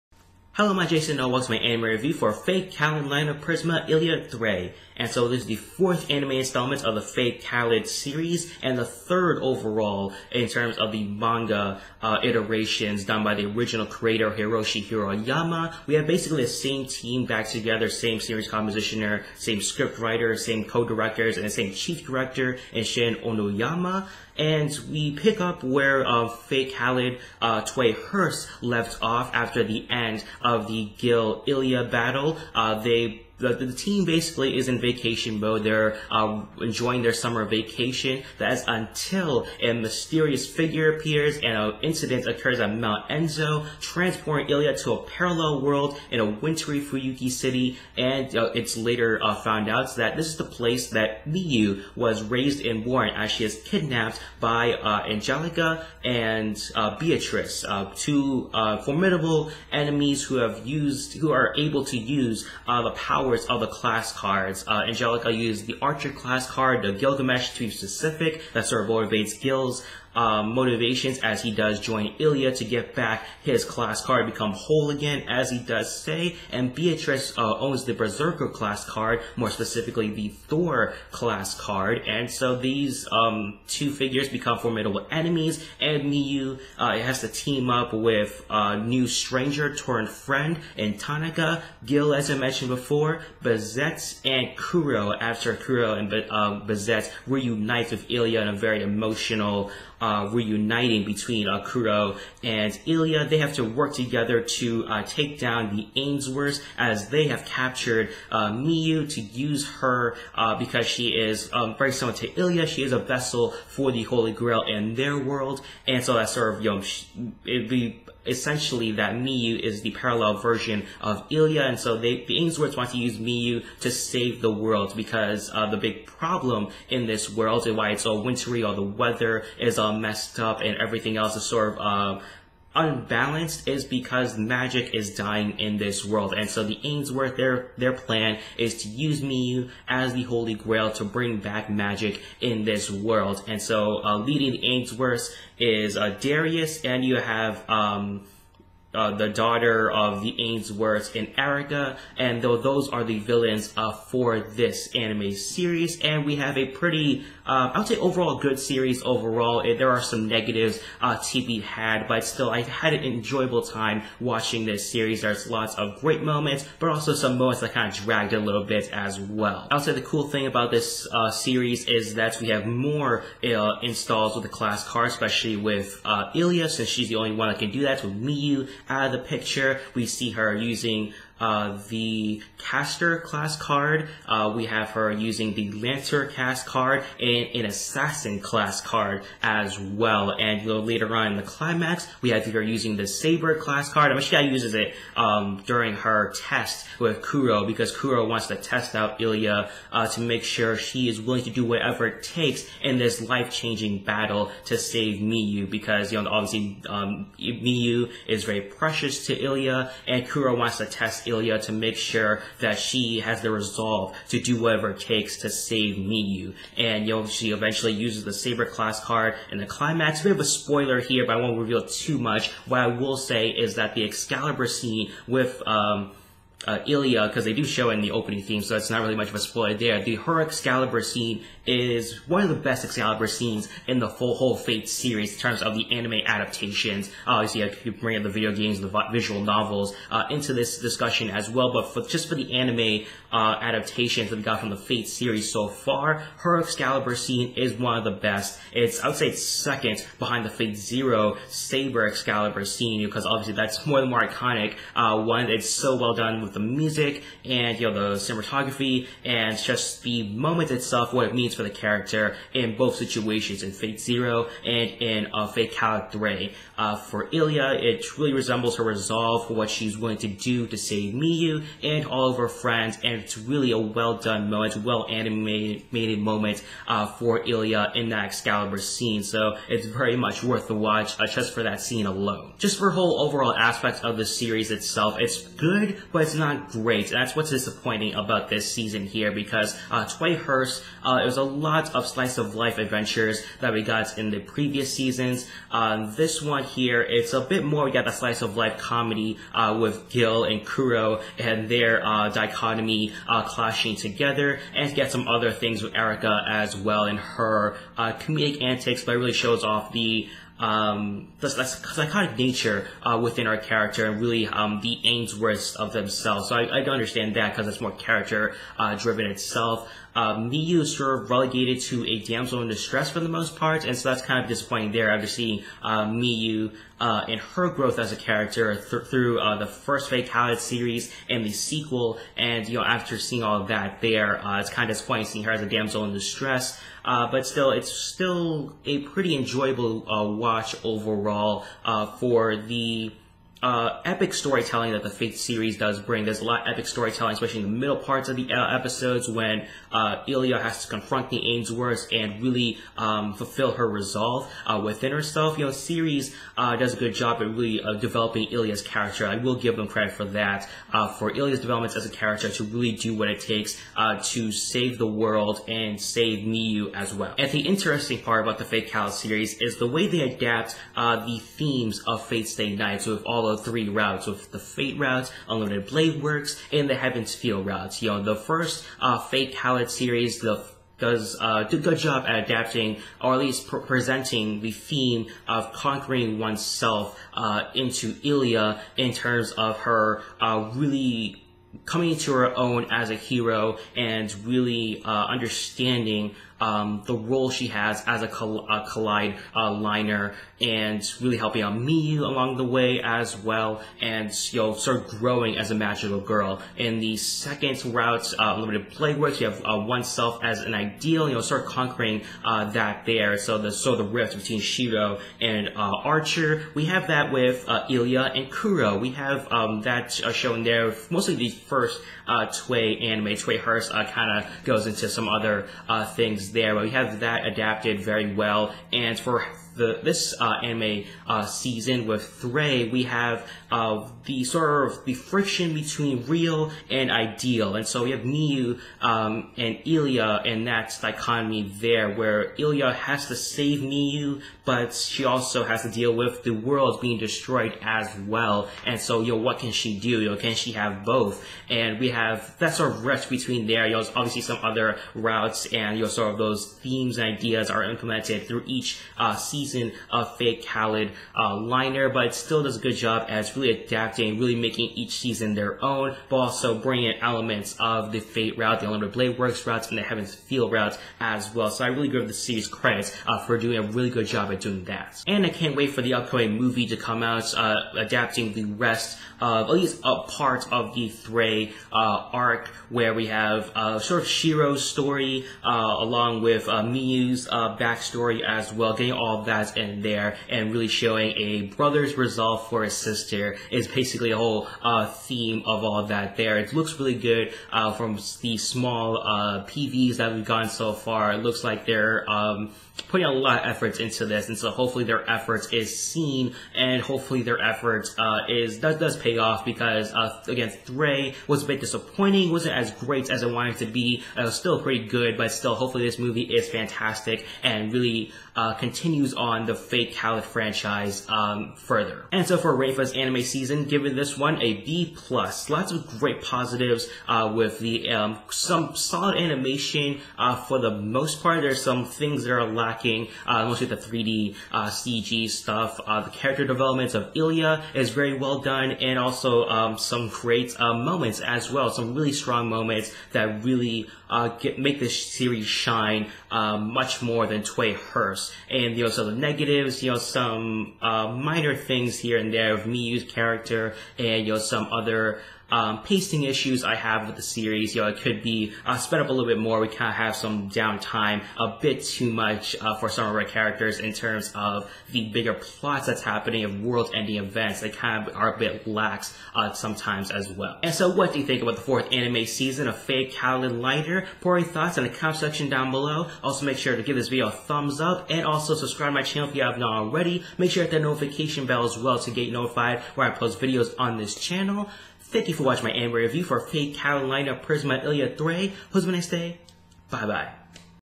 Fate/kaleid liner Prisma Illya 3rei!! Anime Review
In anticipation for the film Gekijōban Fate/kaleid liner Prisma Illya: Sekka no Chikai I review the preceding TV anime series of the franchise Fate/kaleid liner Prisma Illya 3rei!!